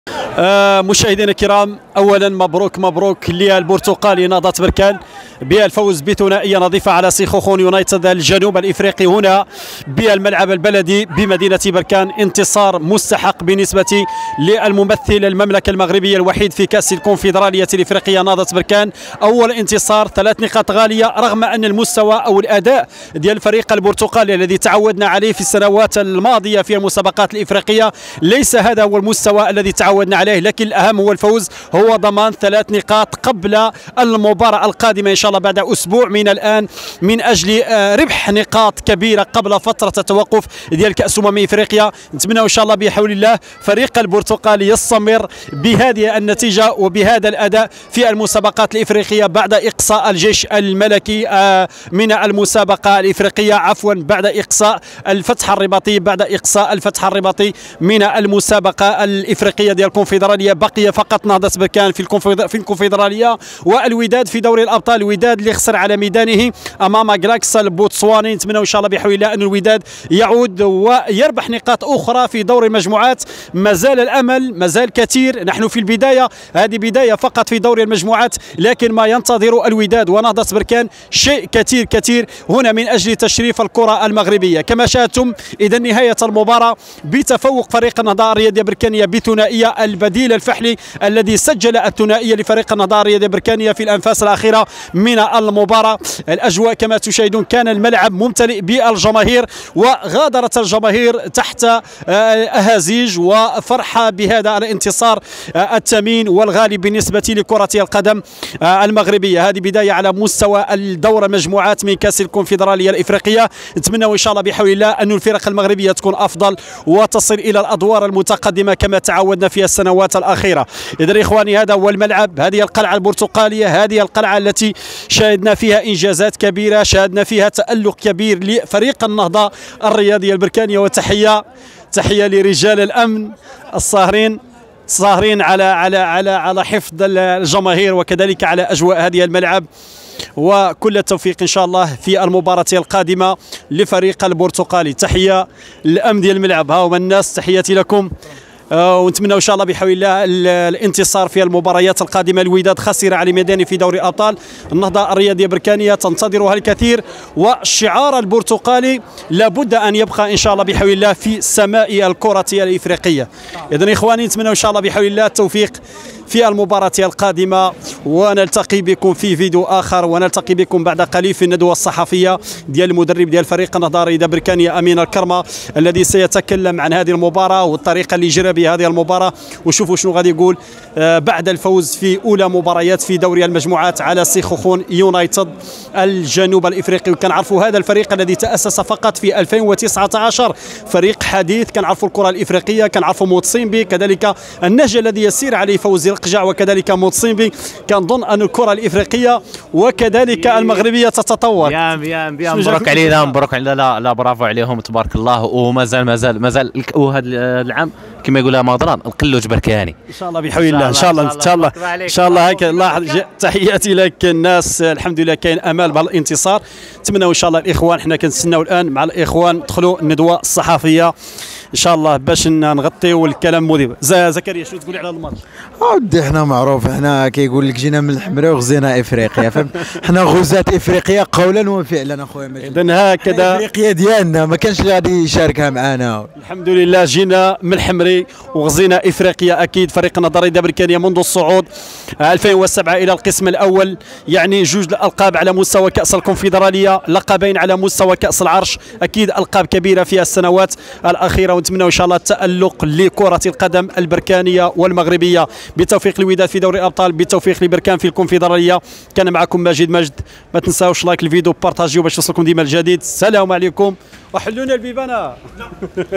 فريق مغربي ديما مشرف المغرب نهضة بركان يفوز على سخوخون يونايتد الجنوب الإفريقي
دوري المجموعات لكأس الكنفدرالية
فوز نهضة بركان على سخوخون يونايتد